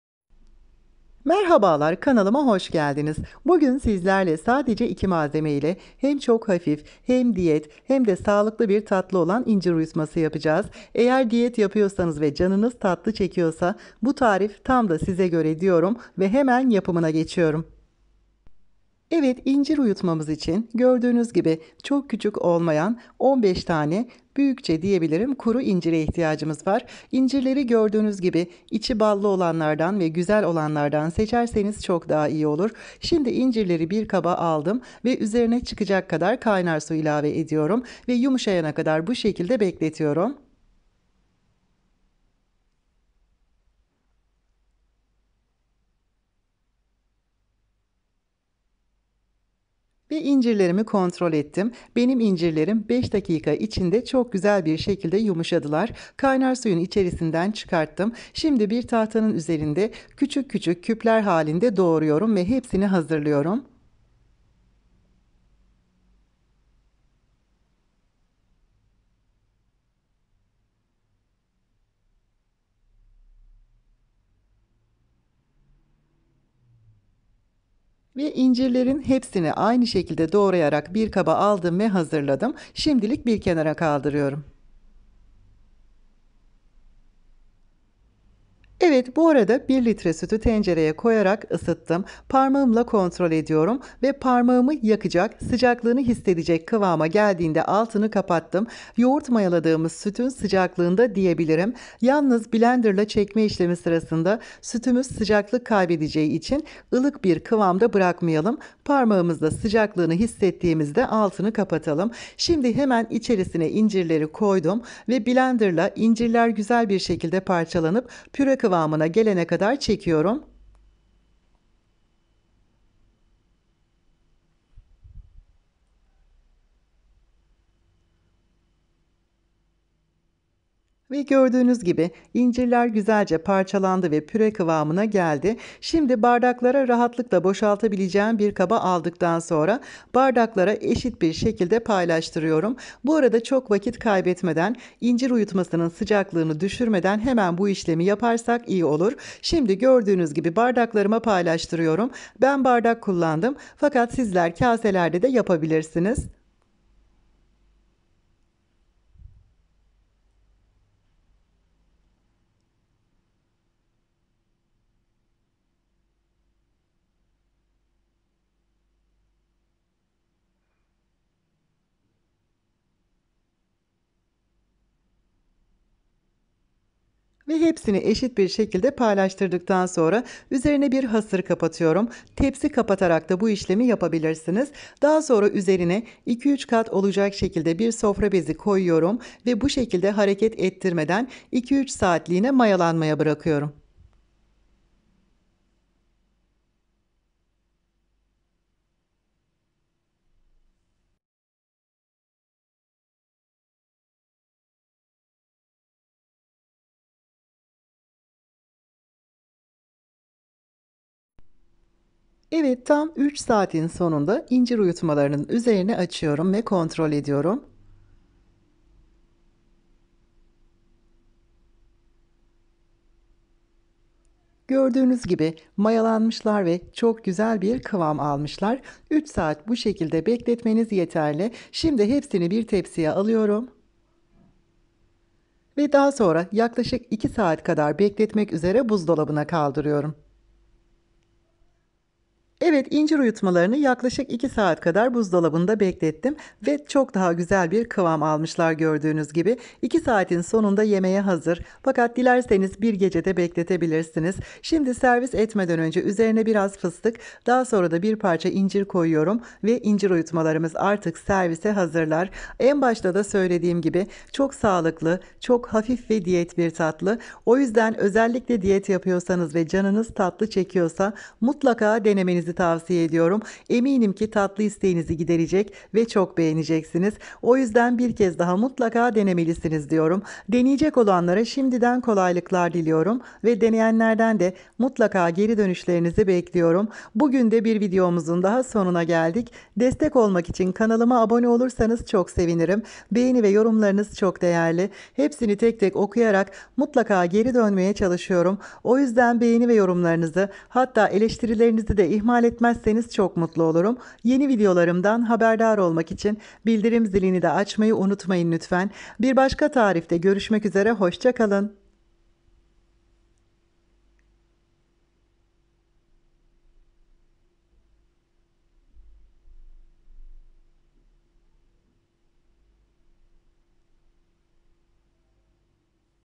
SADECE 2 MALZEME! SIFIR ŞEKER! HEM DİYET HEM HAFİF VE LEZZETLİ BİR TATLI İNCİR UYUTMASI TARİFİ 💯👌
MALZEMELER
15 tane kuru incir
1 lt süt